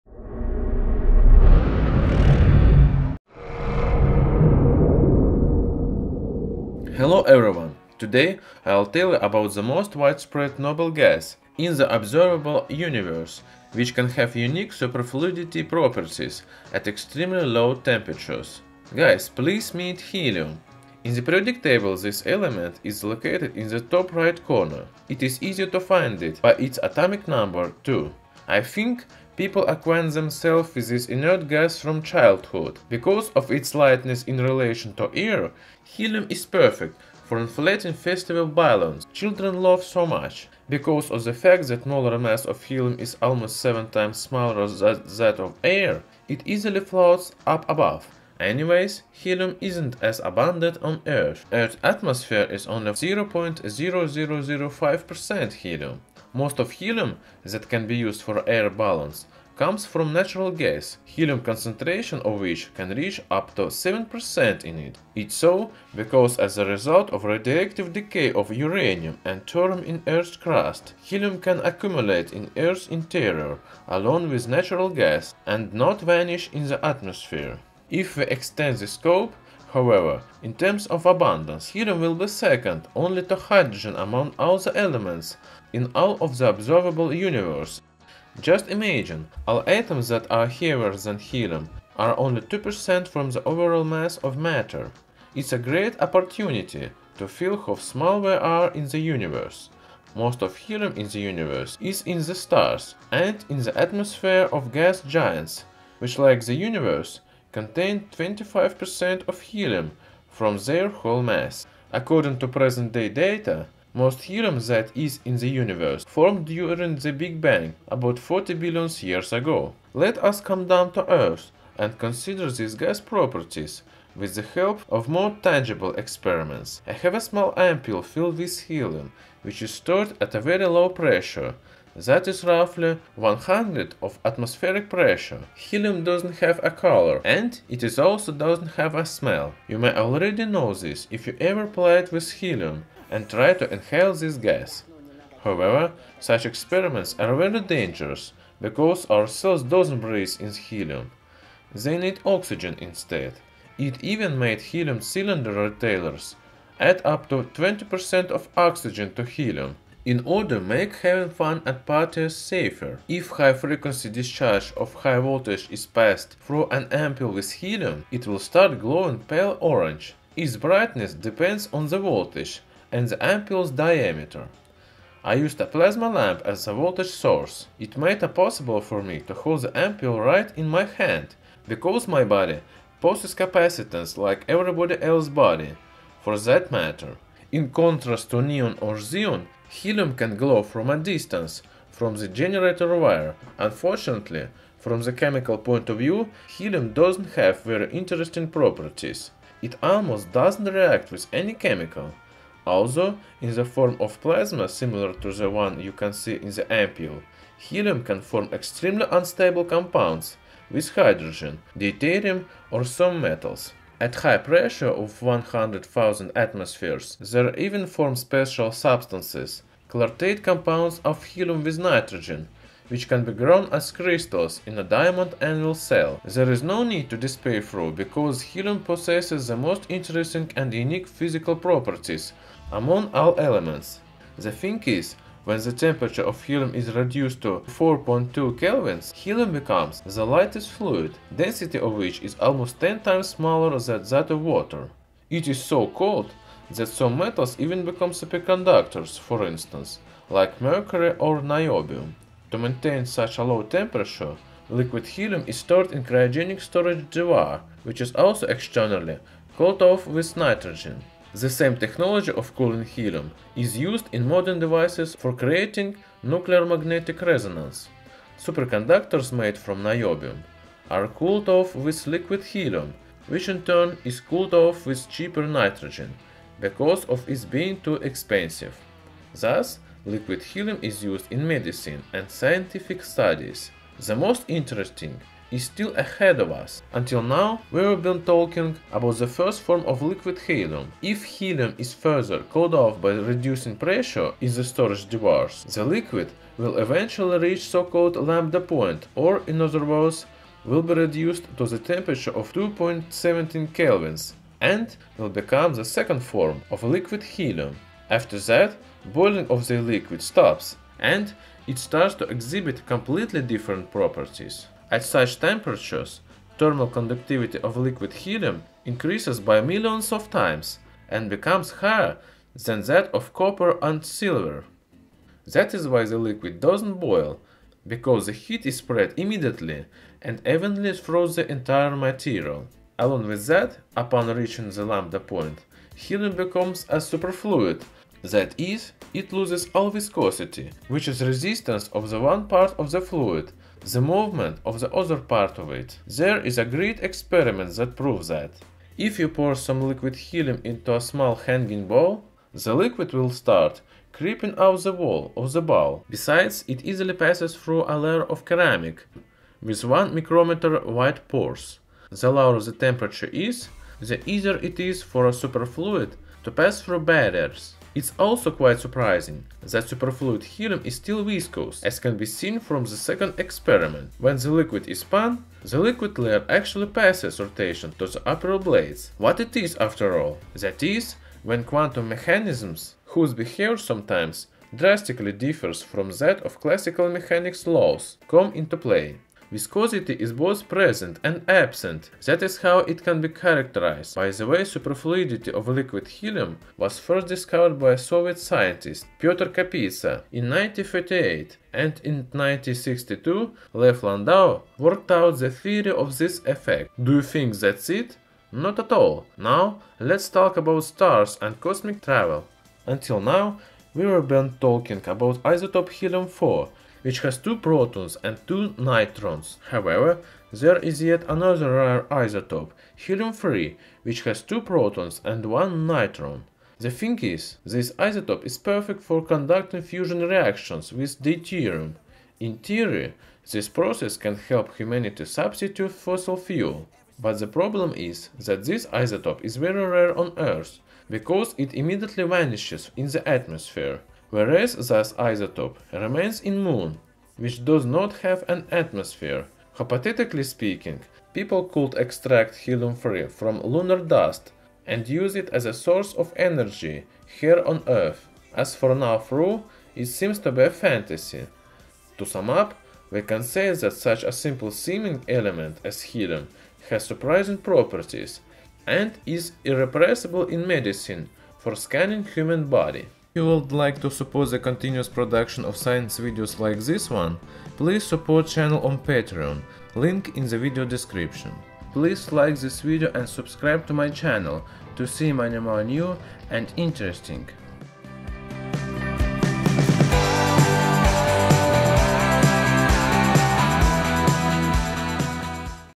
Helium - A SUPERFLUID Element, THAT CAN CLIMB WALLS!
Do not repeat the experiments shown in this video!
Today I will tell you about the most widespread noble gas in the observable universe which can have unique superfluidity properties at extremely low temperatures. Guys, please meet helium. In the periodic table this element is located in the top right corner. It is easy to find it by its atomic number 2. I think people acquaint themselves with this inert gas from childhood. Because of its lightness in relation to air, helium is perfect for inflating festive balloons children love so much. Because of the fact that molar mass of helium is almost 7 times smaller than that of air, it easily floats up above. Yes, exactly it floats because from the point of view of fluid dynamics, fluids and gases exhibit similar properties.  Earth’s atmosphere loses about 50 grams of helium a second because of the extreme lightness of this gas. Anyways, helium isn’t as abundant on earth. Earth's atmosphere is only about 5 parts per million, or 0.0005 percent, helium. Most of helium that can be used for air balloons comes from natural gas helium concentration of which can reach up to 7 percent in it! It’s so because as a result of radioactive decay of uranium and thorium in earth’s crust, helium can accumulate in earth’s interior along with natural gas and not vanish in the atmosphere.